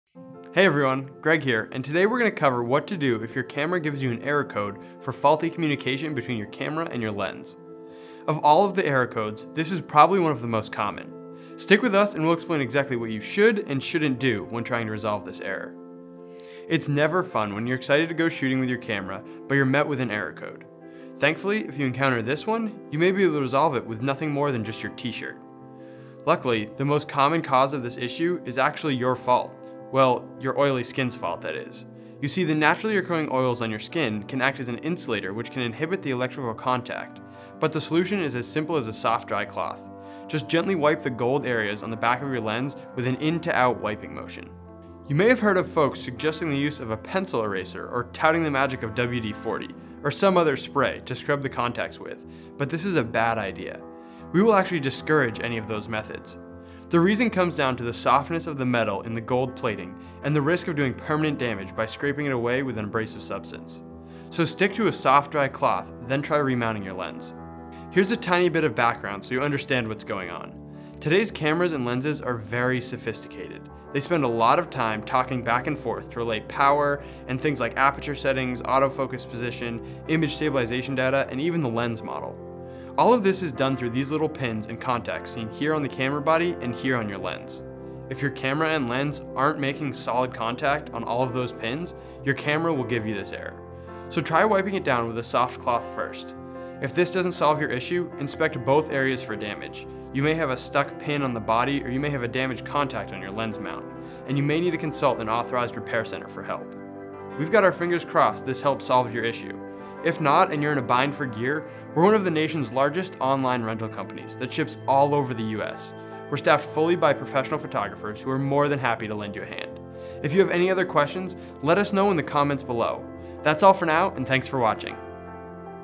How to fix lens communication error on Sony cameras
If your Sony DSLR or mirrorless camera is giving you an error when you attach a suitable lens, this one trick might solve your problem.

Music from ArtList IO - Get 2 months FREE!